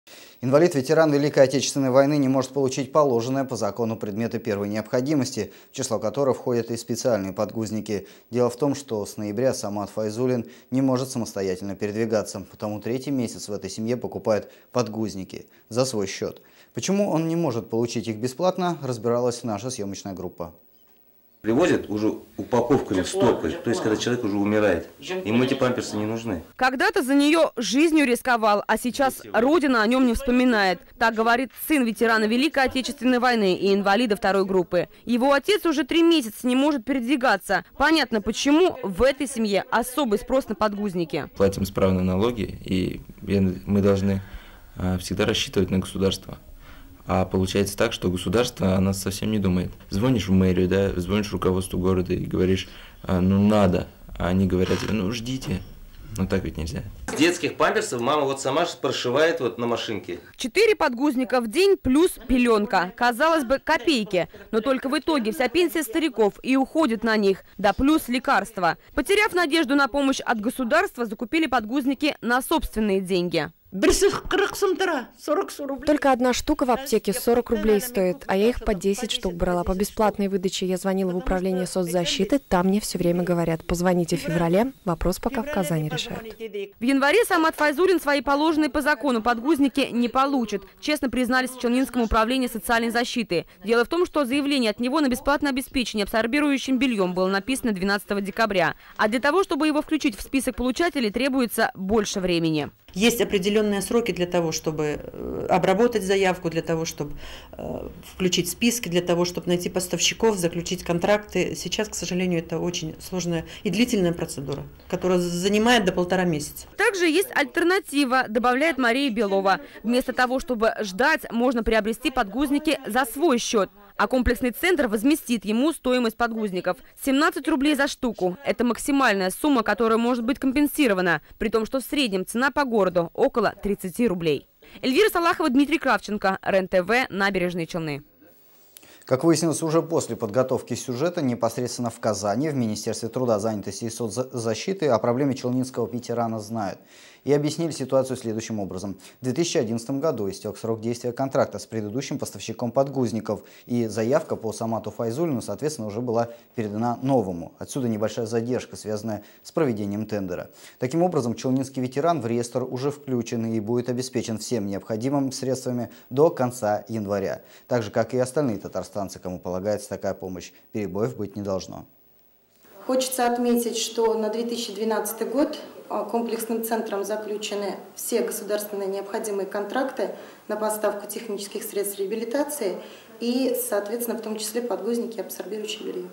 Ветеран не может получить положенное по закону
Инвалид, ветеран Великой отечественной войны не может получить положенные по закону предметы первой необходимости, в число которых входят и специальные подгузники